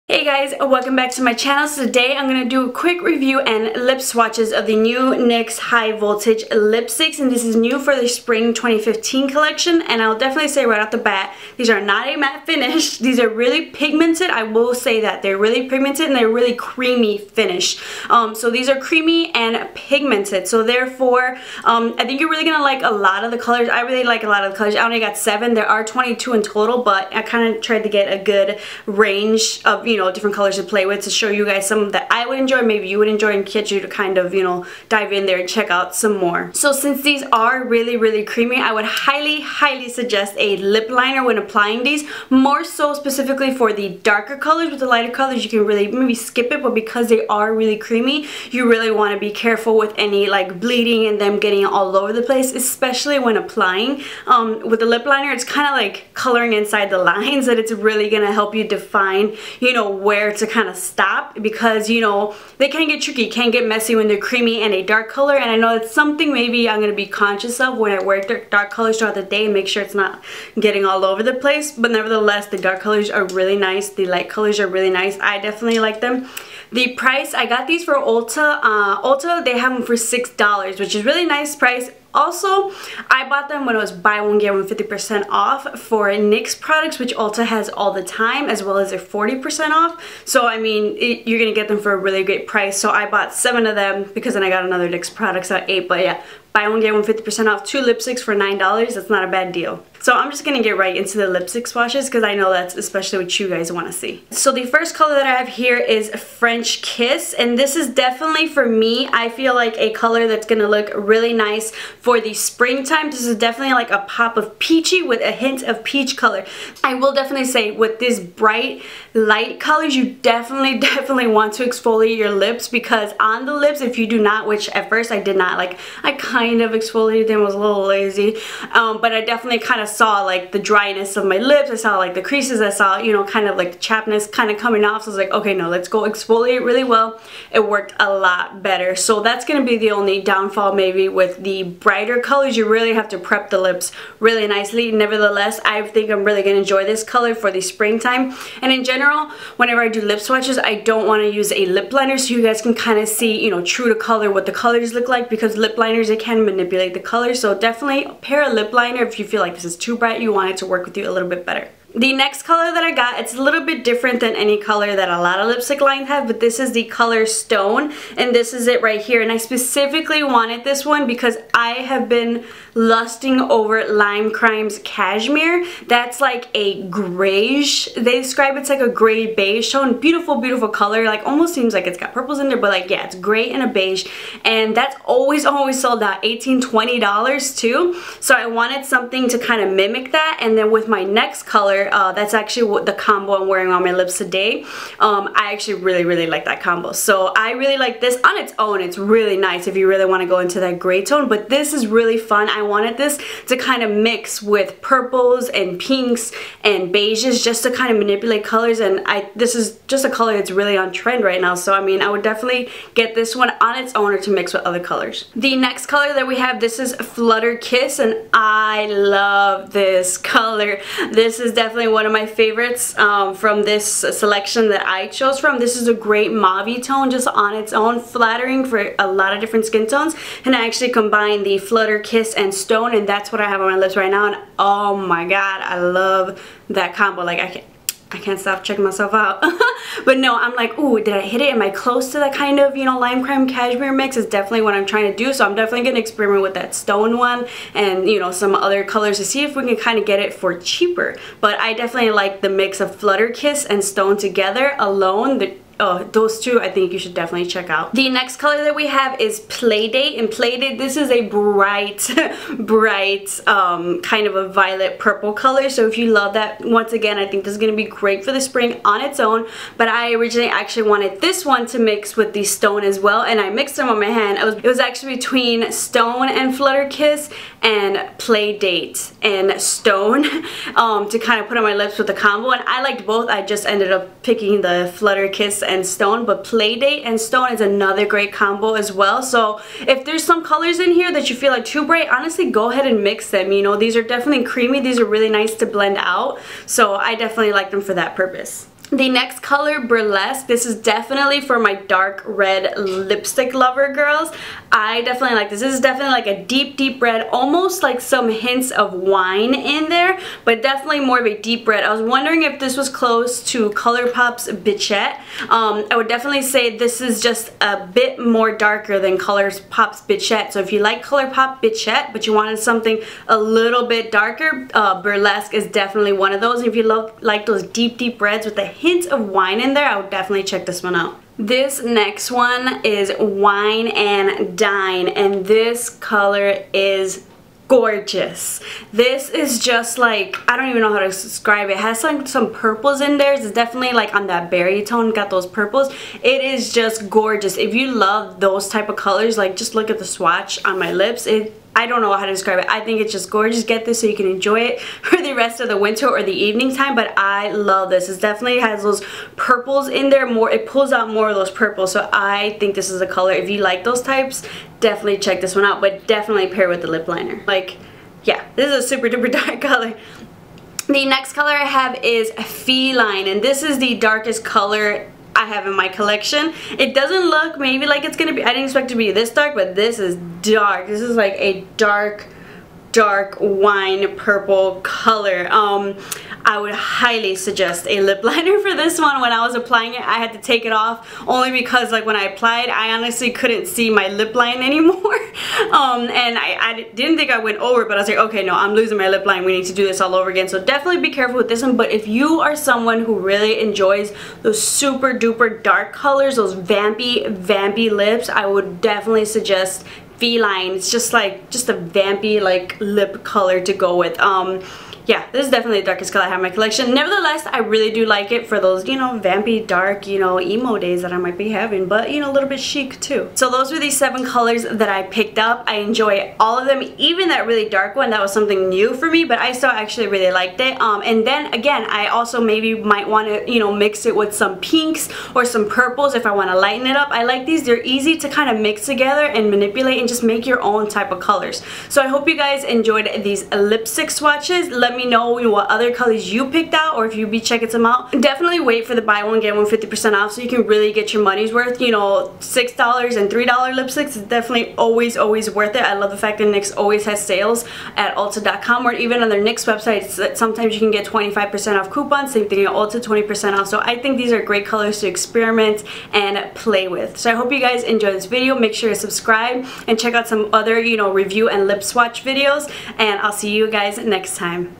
NYX High Voltage Lipsticks + Lip Swatches
I had to re-upload this video. Technology has been against me this last week. First my iMovie, Mac, iPhoto then Youtube processing :/...Hope you Enjoy!

Lip Colors Swatches:
French Kiss
Stone
Flutterkiss
Play Date
Burlesque
Wine and Dine
Feline

What I'm Wearing:
Face: Maybelline Fit Me Foundation Dewy + Smooth
Cheeks: Mac Blush Baby
Eyes: Sonia Kashuk Eye on Neutral Matte Palette
Lips: NYX High Voltage Lipstick Flutter Kiss + Stone
Nails: Essie French Affair